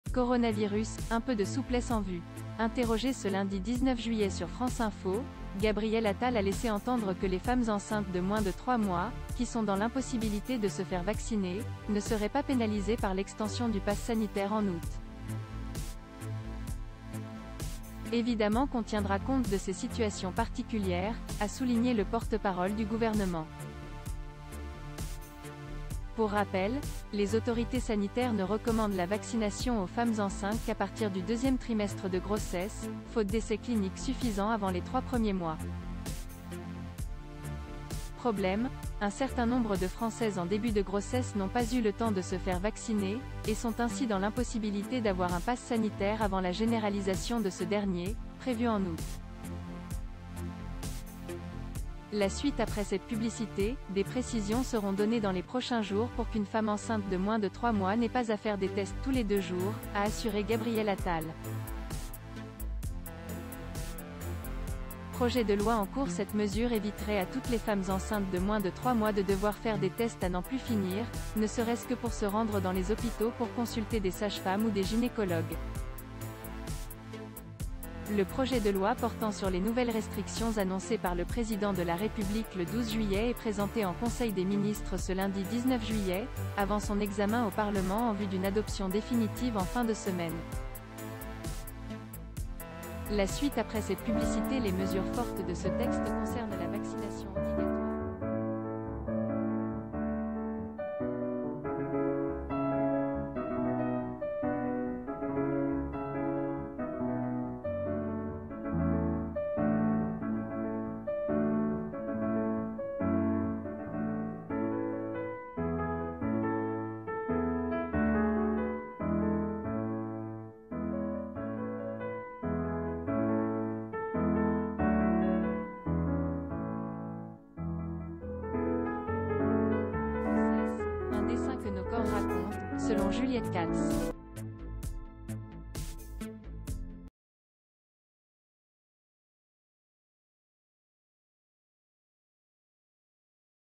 L'objectif: éviter qu'une femme enceinte de moins de 3 mois soit contrainte de se faire tester tous
L'objectif: éviter qu'une femme enceinte de moins de 3 mois soit contrainte de se faire tester tous les deux jours.